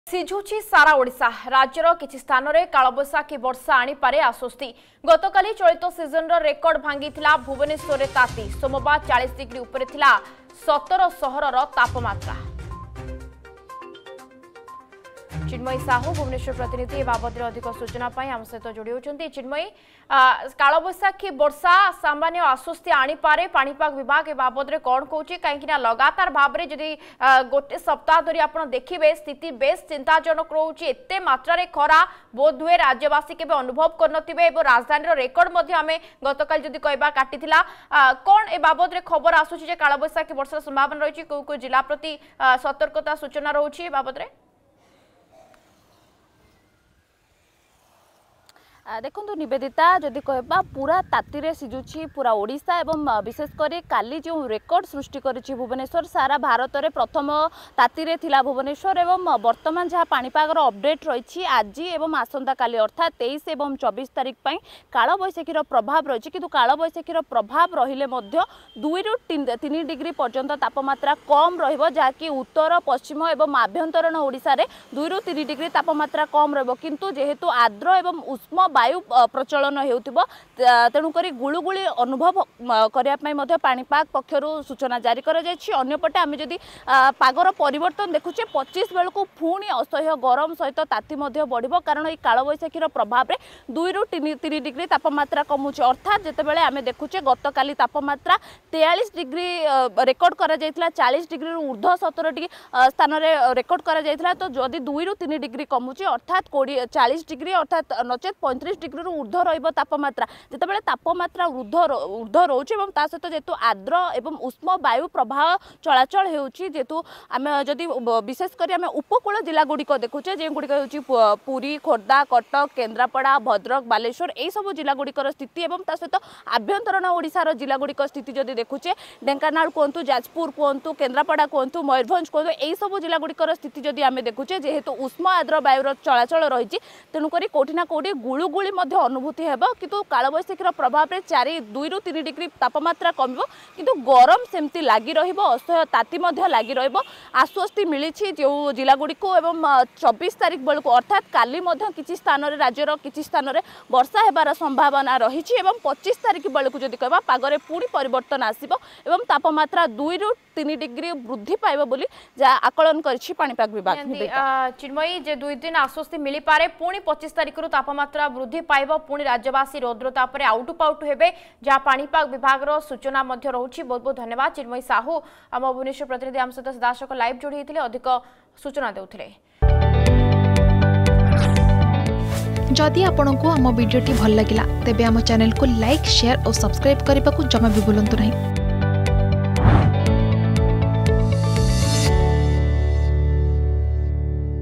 ସିଝୁଛି ସାରା ଓଡ଼ିଶା || Summer weather update
ସିଝୁଛି ସାରା ଓଡ଼ିଶା । ରାଜ୍ୟର କିଛି ସ୍ଥାନରେ କାଳବୈଶାଖୀ ବର୍ଷା ଆଣିପାରେ ଆଶ୍ବସ୍ତି । ଗତକାଲି ଚଳିତ ସିଜିନରେ ରେକର୍ଡ ଭାଙ୍ଗିଥିଲା ଭୁବନେଶ୍ବରରେ ତାତି ।